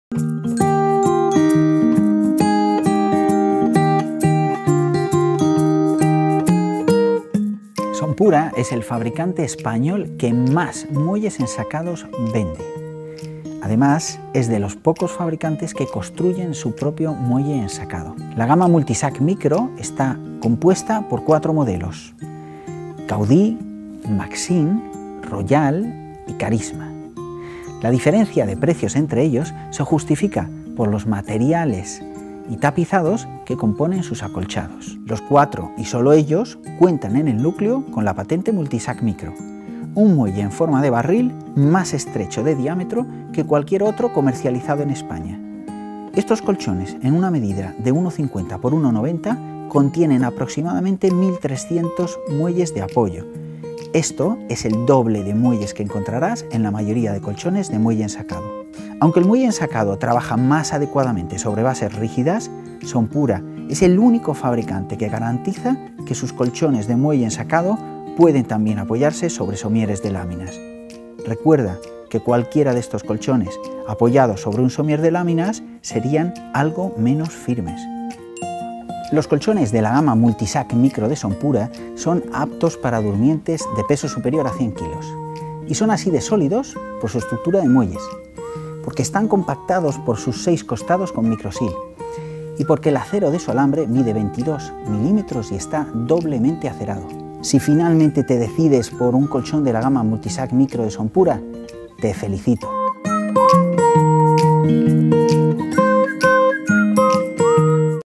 muelles ensacados Multisac Micro Sonpura
Conoce de qué está formado el núcleo de muelles ensacados de la gama Multisac Micro de Sonpura. Este fabricante incorpora más de 1300 muelles embolsados de manera independiente en colchones de 150x190. Podrás ver cómo trabaja su flexibilidad y qué modelos de los disponibles en el mercado actualmente cuentan con este sistema.
(Nota: en febrero de 2016 cambiaron las tapicerías y composiciones de estos colchones. El Maxim ya no existe y los otros han cambiado su tapicería y, en algo, su composición, para ver los modelos actualizados tienes que ir a la web).